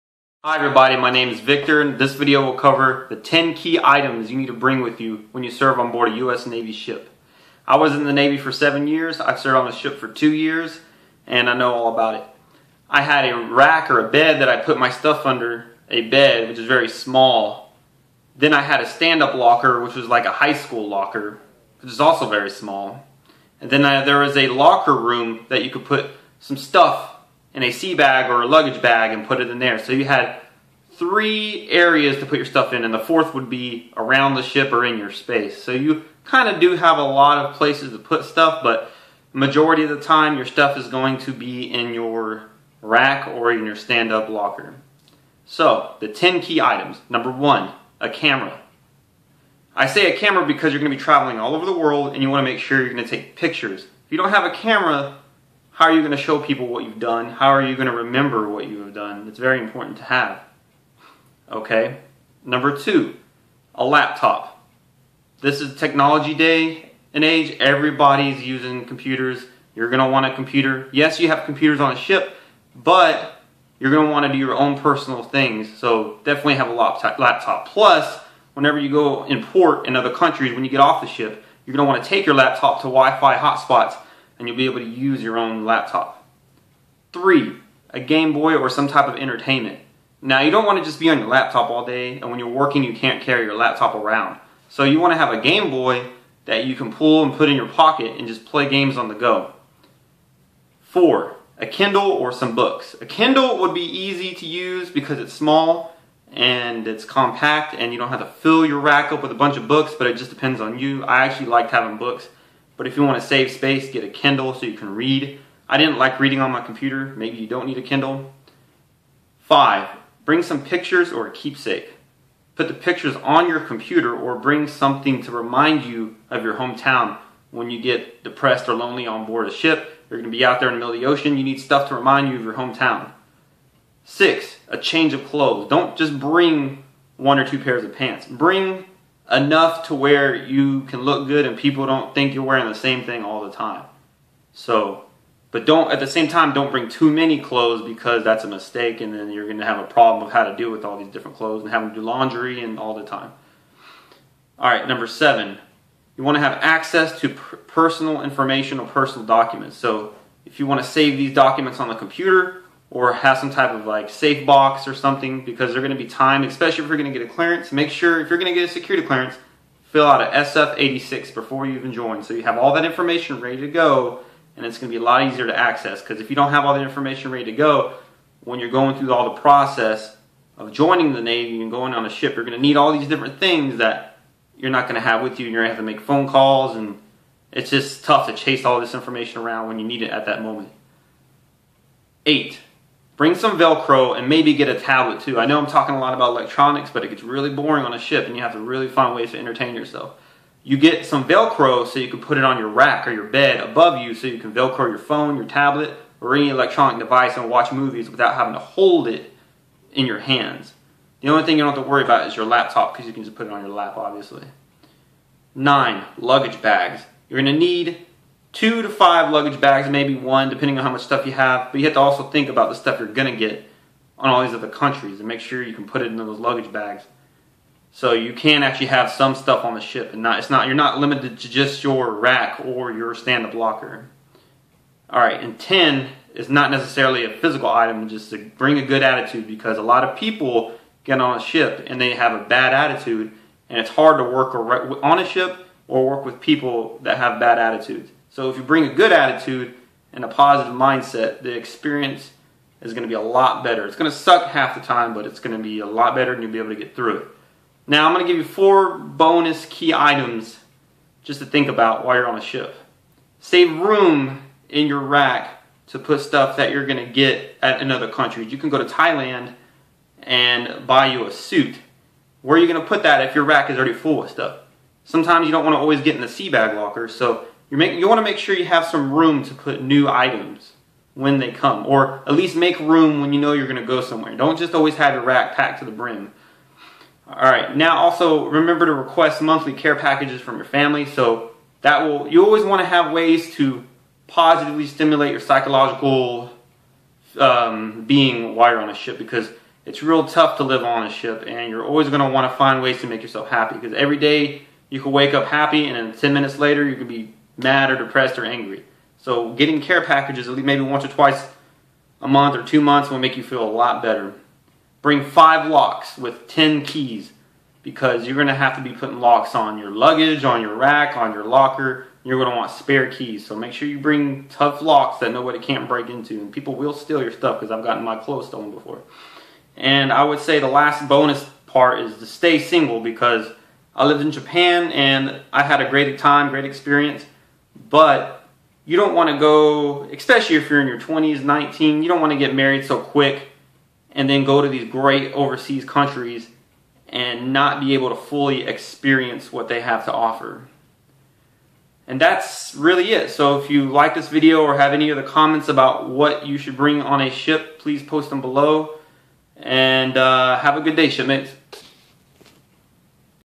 Navy - 10 Essential items to bring on board
Hello Everyone. This video covers the 10 key items you need to bring with you while you are serving on board a US Navy, or any Navy Ship. Enjoy!